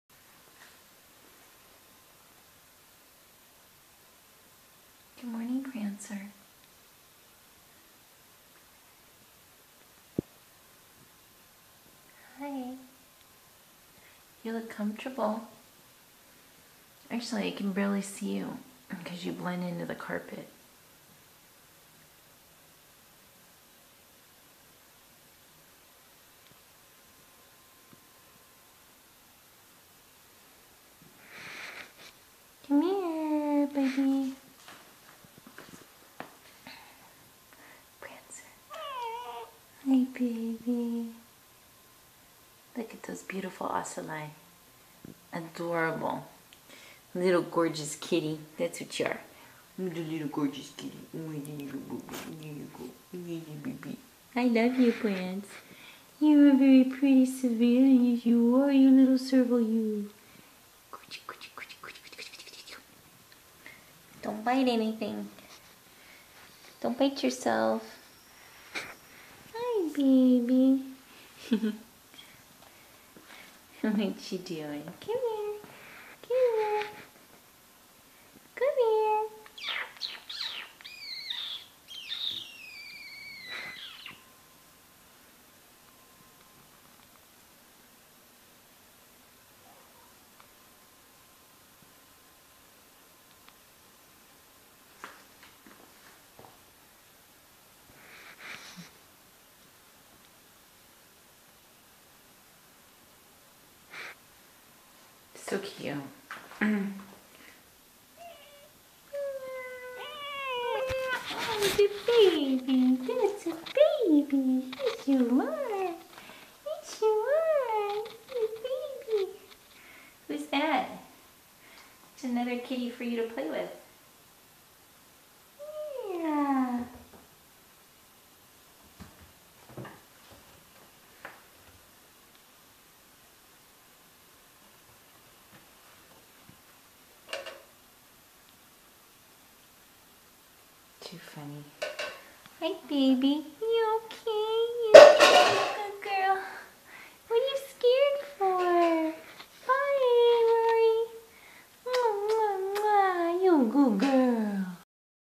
Good Morning Prancer - A Savannah Headbutt
Good morning Prancer! Our beautiful F1 Savannah boy

👉👉 Check out the supplements that we use for all of our exotic cats and kittens. and use code 49078 when placing your order!!!